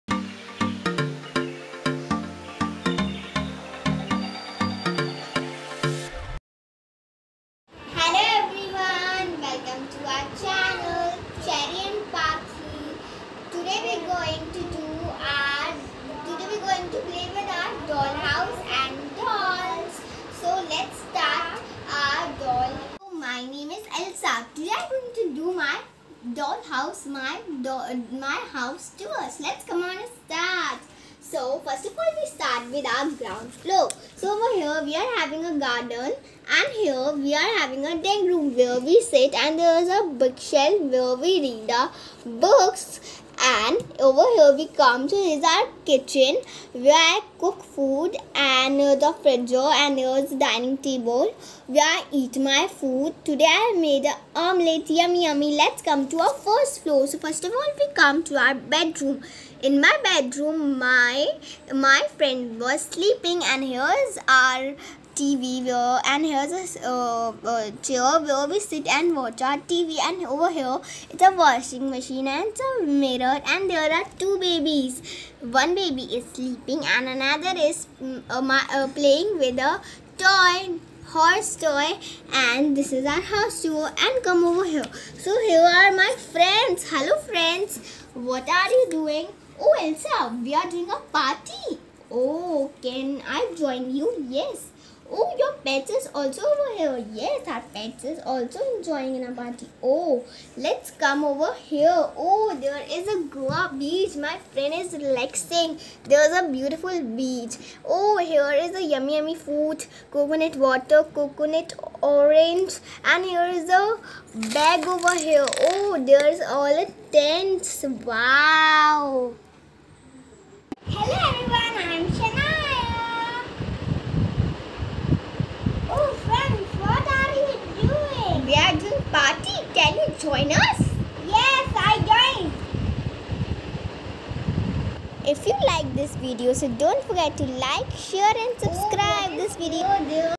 Our Doll house tour#Playing with doll's#Fun & Enjoy 🏖️🏫
A doll house is a toy homemade in miniature .contemporary children's play Doll house a mostly in two third scale.